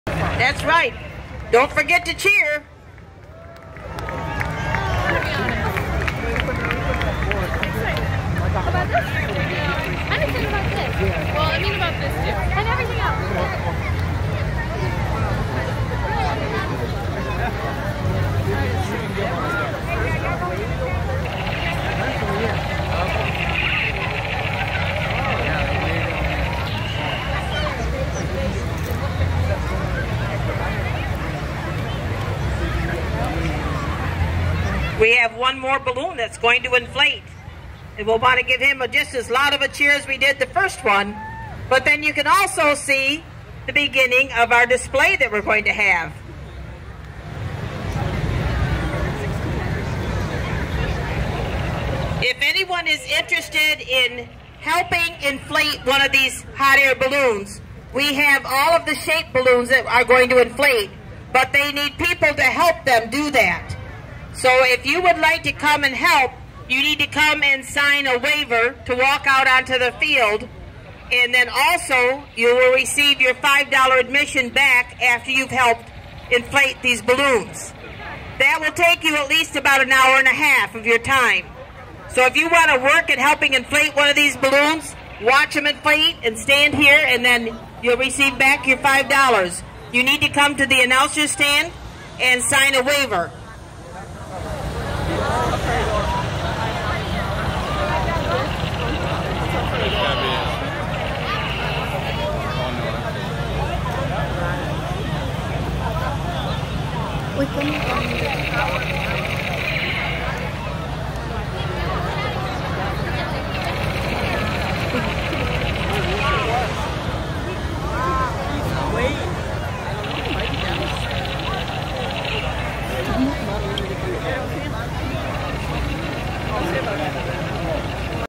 Hot Air Balloon Festival!
This was last yr! Hot Air Balloon Festival @ Dansville